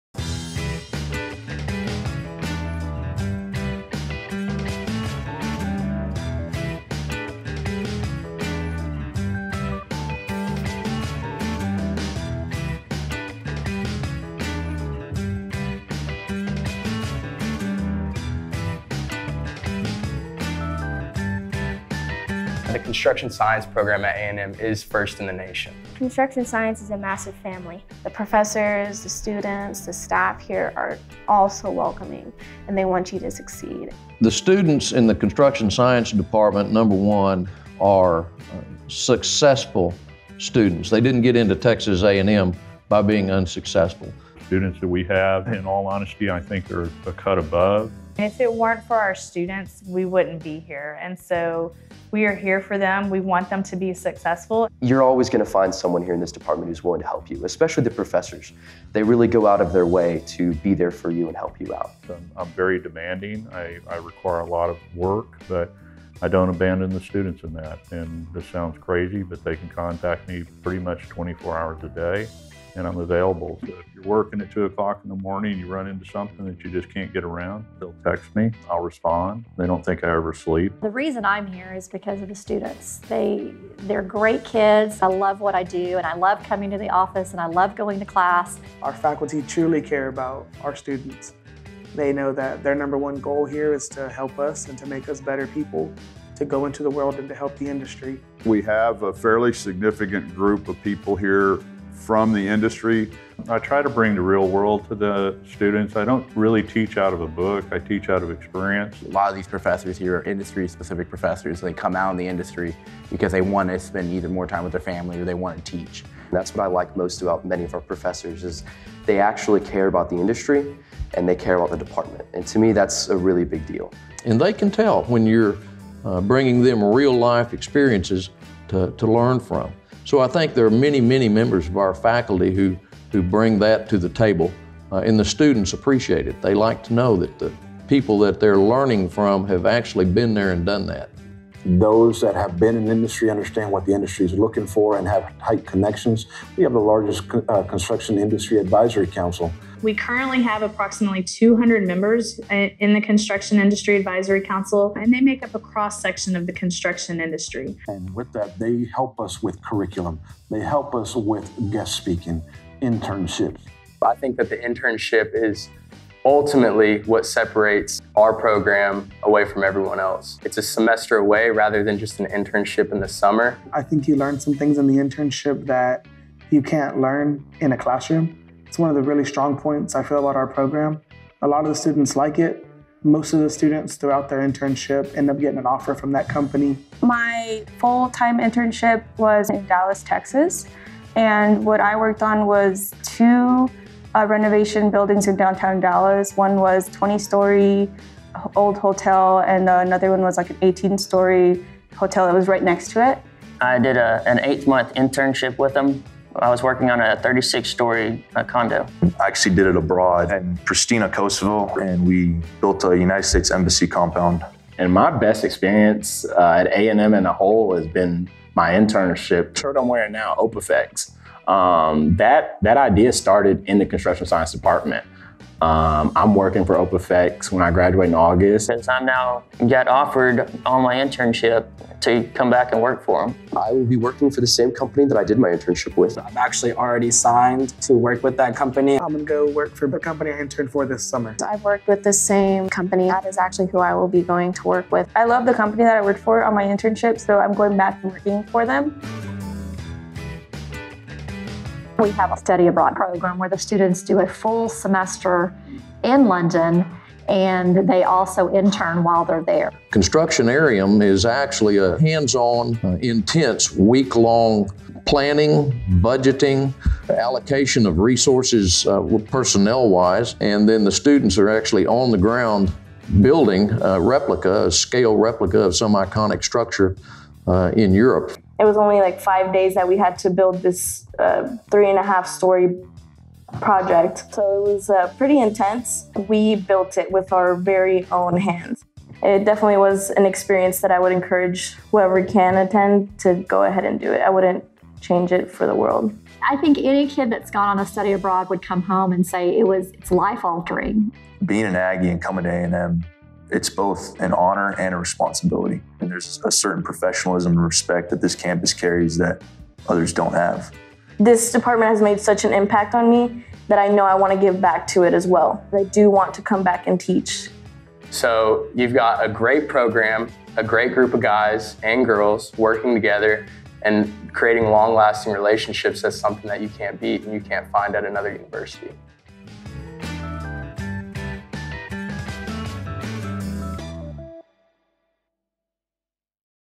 Learn about the Texas A&M Construction Science Department
The Construction Science Department is dedicated to education, discovery, development and application of knowledge in the field of construction while fulfilling the land grant mission of Texas A&M University and enhancing the economic development of the State of Texas. Our mission of providing the highest quality academic programs is inseparable from our mission of developing new understanding through teaching, research and service. We prepare students to assume roles in leadership, responsibility and service to society.